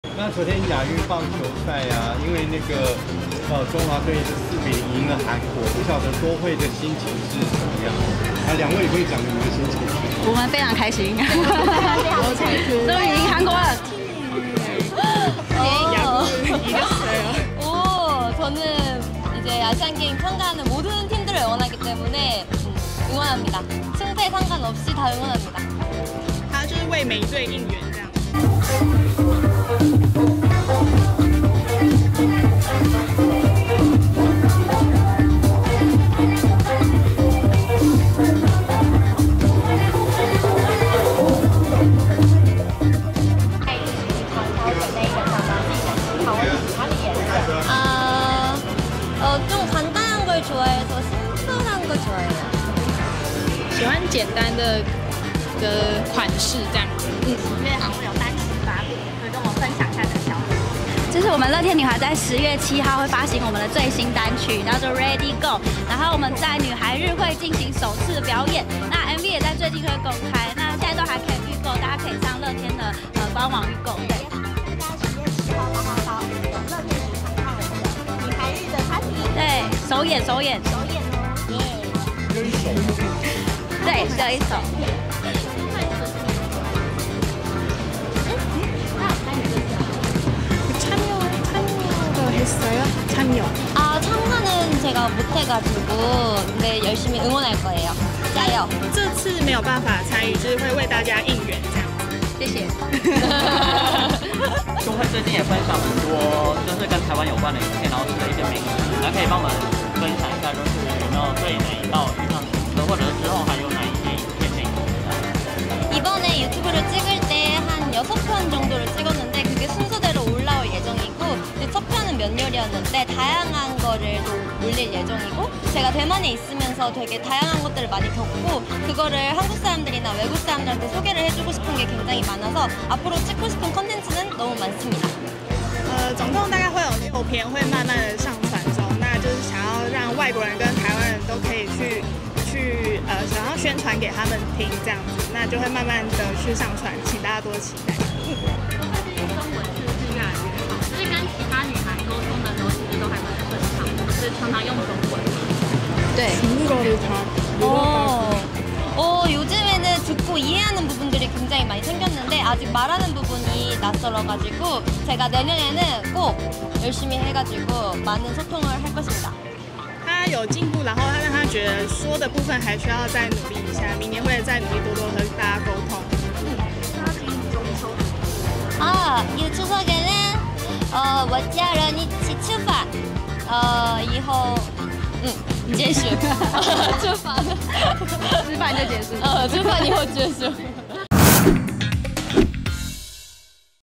亞運棒球4：0戰勝韓國 李多慧甜笑回應超得體｜壹蘋新聞網
【亞運棒球台灣4:0完封韓國　李多慧給出超得體回應】